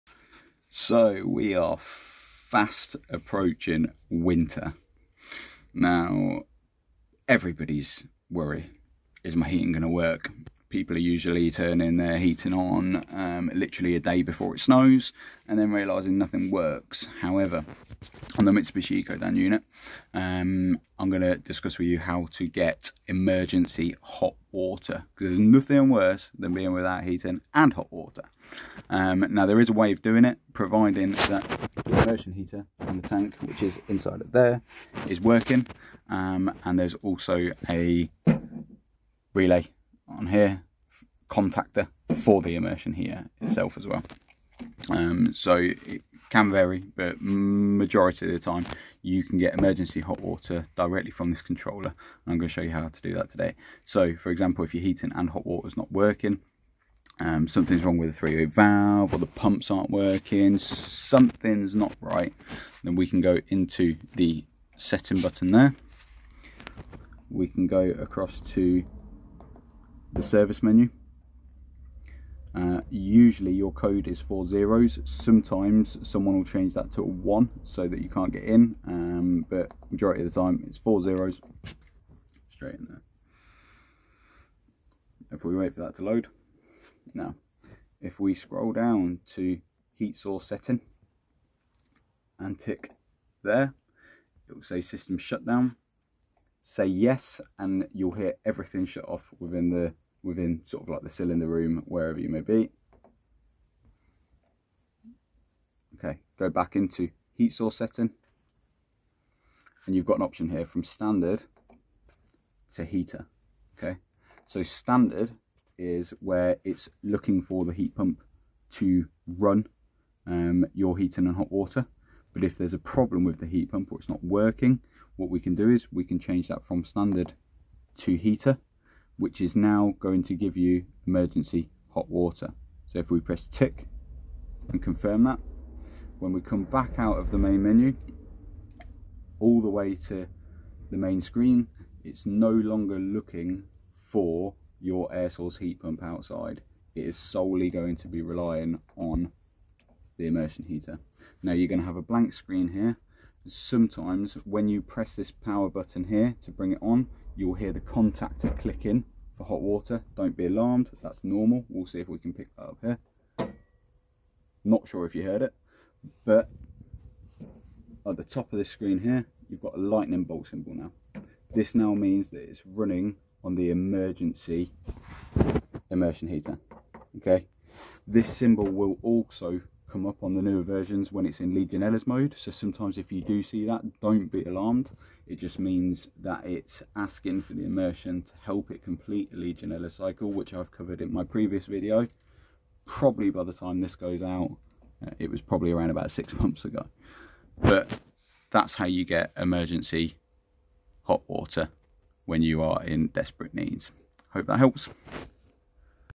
no hot water Mitsubsihi Ecodan, Emergency mode
In this video il show you how to get emergency hot water in the event of nothing working on your air source heat pump, this gives you emergency hot water giving your engineer time to get out to you during peak times.